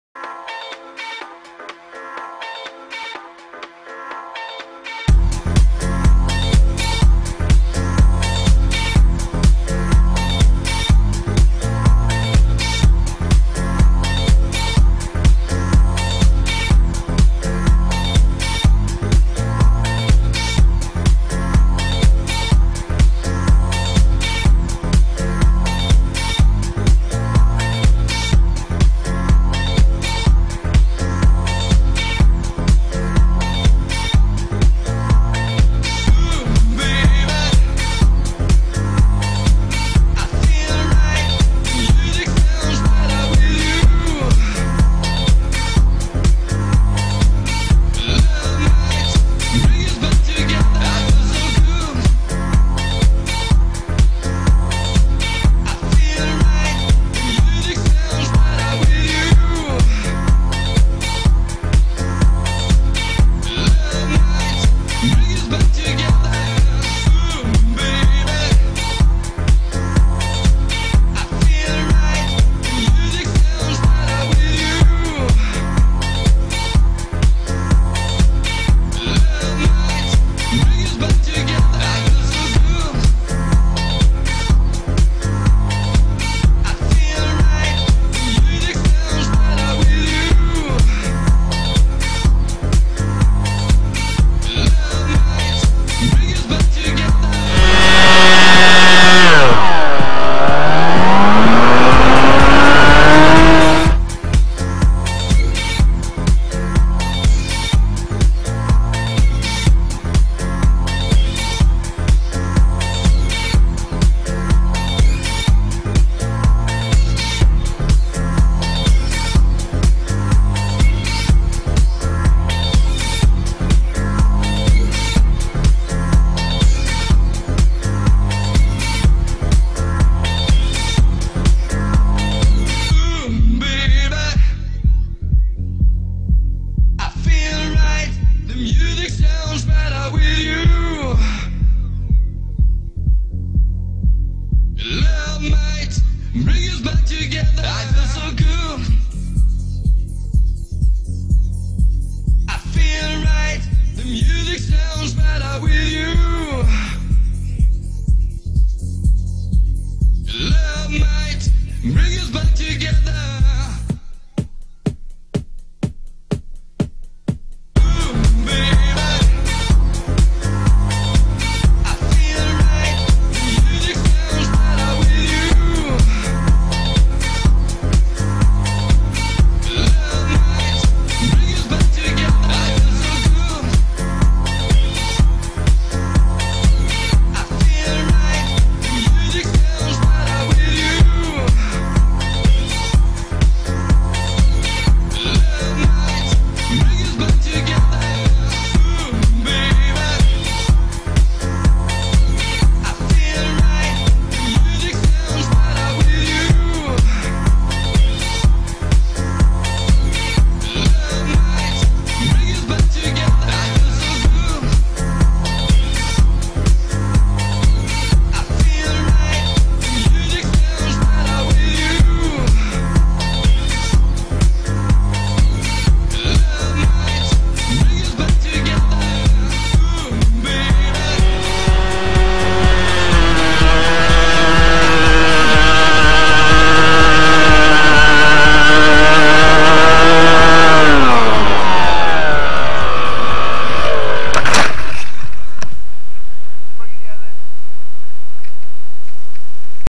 Eflite Beast flying over Dover Green, Reigate
A small cam attached the the Eflite Beast micro